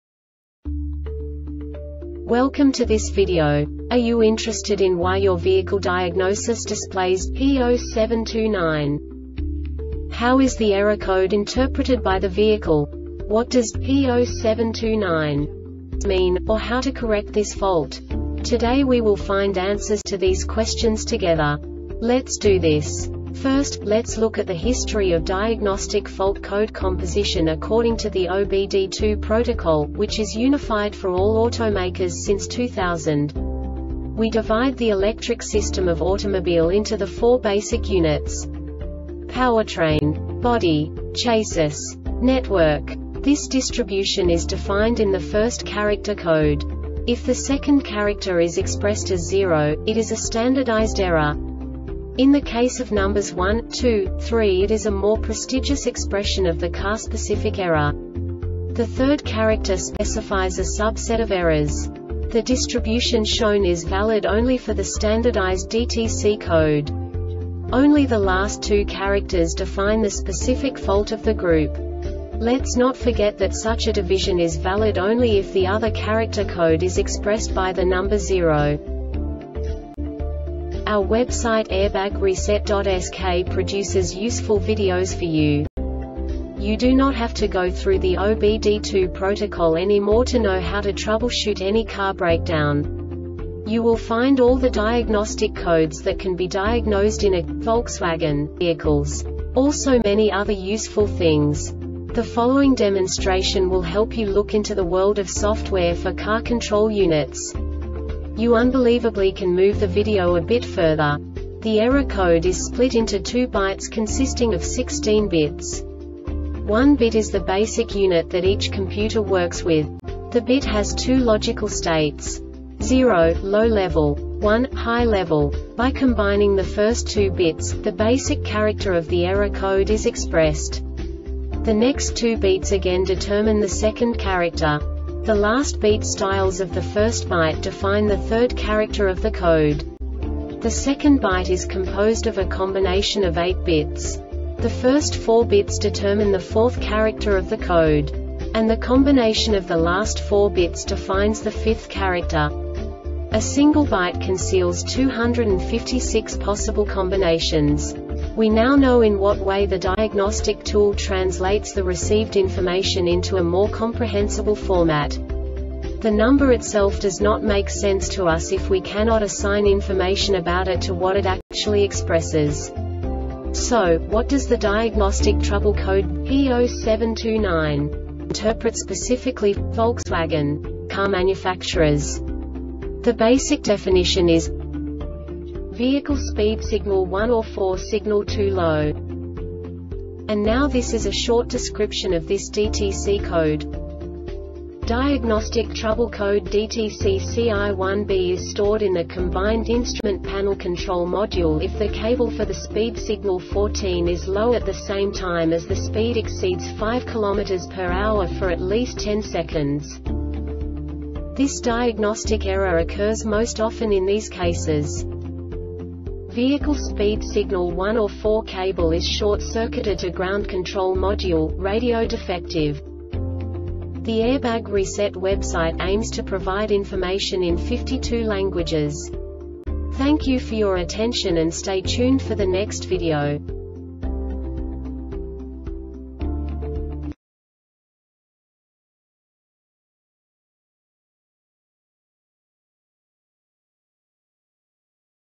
DTC Volkswagen P0729 Short Explanation
Contents:
0:21 Basic DTC analysis according to OBD2 protocol standard.
1:48 Insight into programming
2:58 A diagnostically understandable description of the Diagnostic Trouble Code
3:05 Basic error definition

Make:
Volkswagen
Code:
P0729
Definition:
Vehicle Speed Signal (1 or 4). Signal Too Low
Description:
Diagnostic trouble code (DTC) CI-1B is stored in the combined instrument panel control module if the cable for the speed signal (1 or 4) is low at the same time as the speed exceeds 5 km or h for at least 10 seconds.
Cause:
Vehicle speed signal (1 or 4) cable is short-circuited to ground.Control module,radio defective